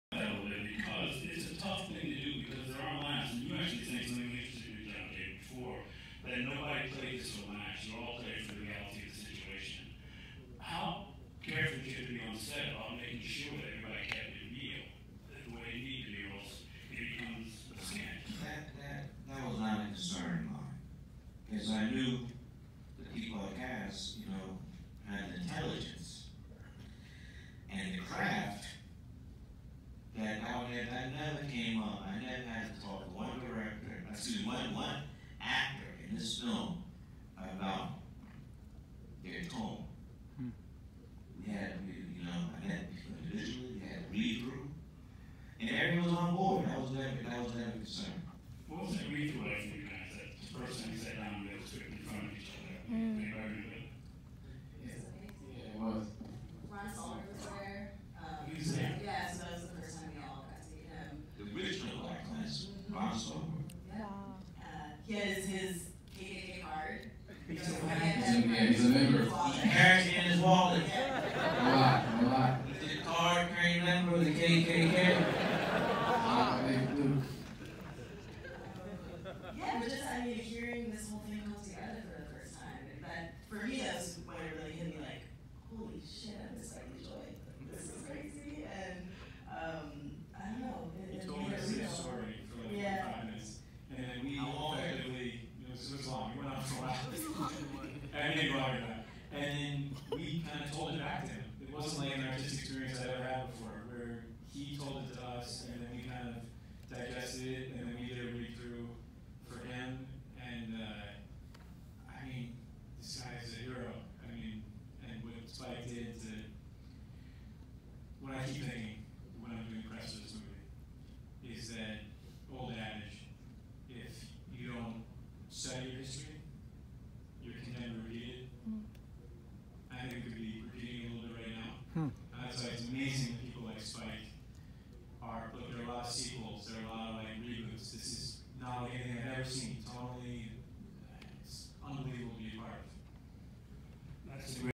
Spike Lee & Cast, Blackkklansman, on allowing his actors their freedom too act & and their initial
Spike Lee & Cast, Blackkklansman q & a @ the Arclight Hollywood 8/8/2018